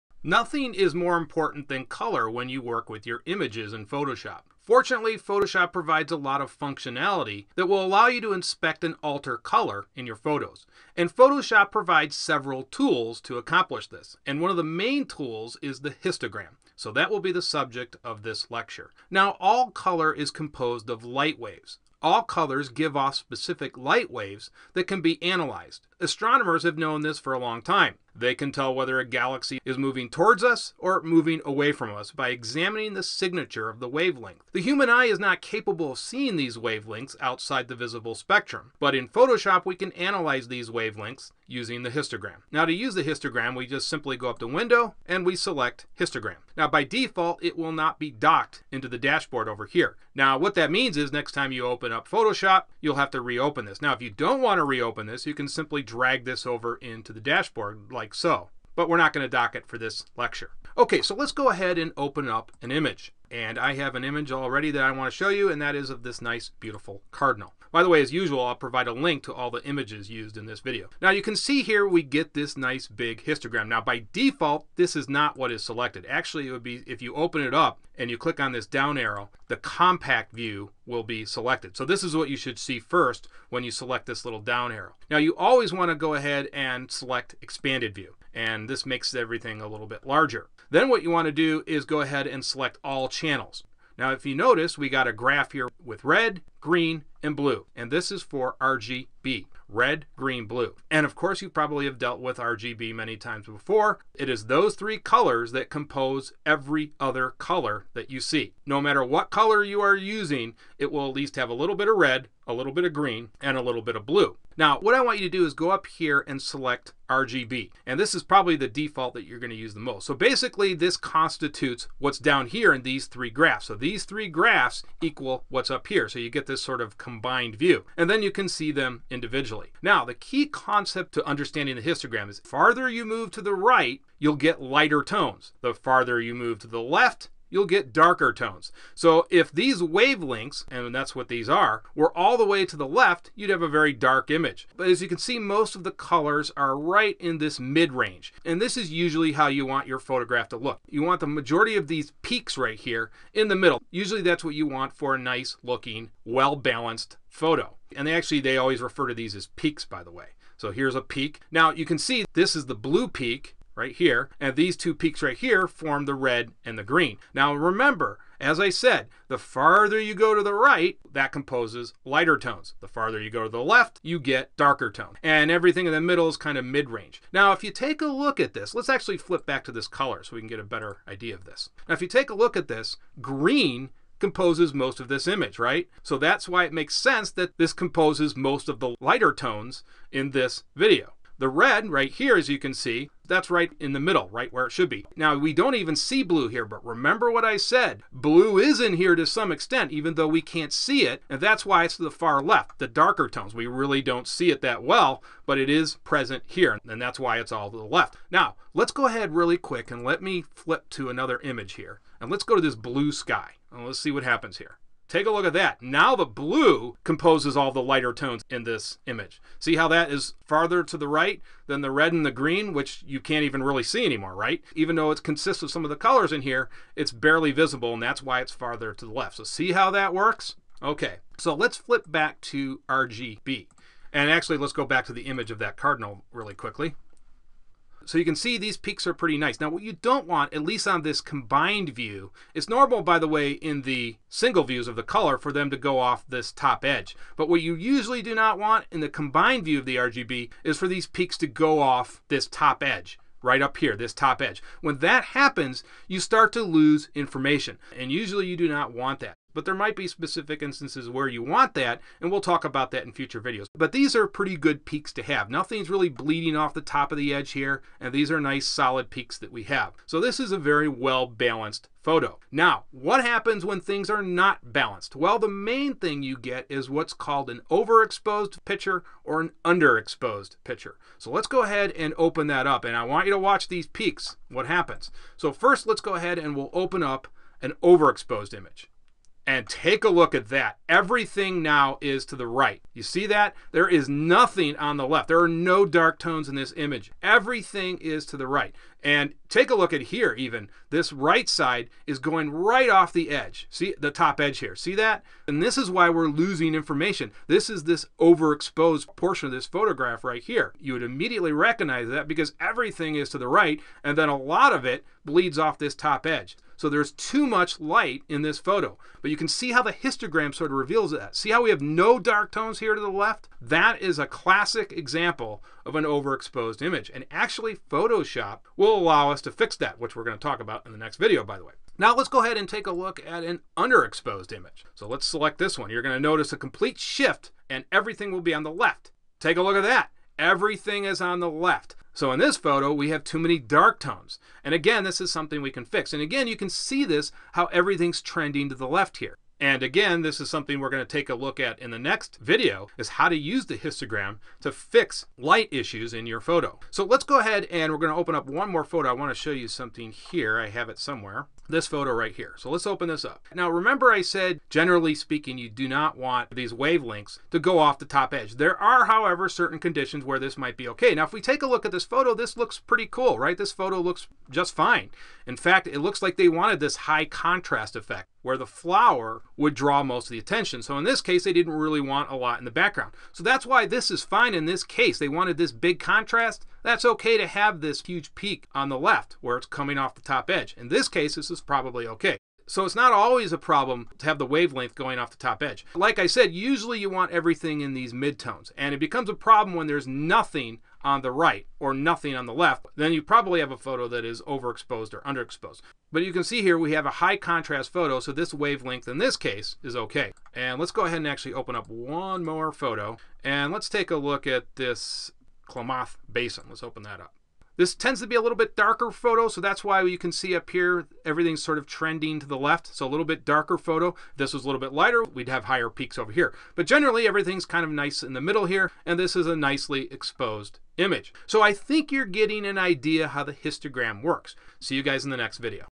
Photoshop Tutorial for Beginners - 02 - Understanding the Histogram
A first look at the Histogram.

Credit for under and overexposed images:
Dawid Skalec / Wikimedia Commons / CC-BY-SA-4.0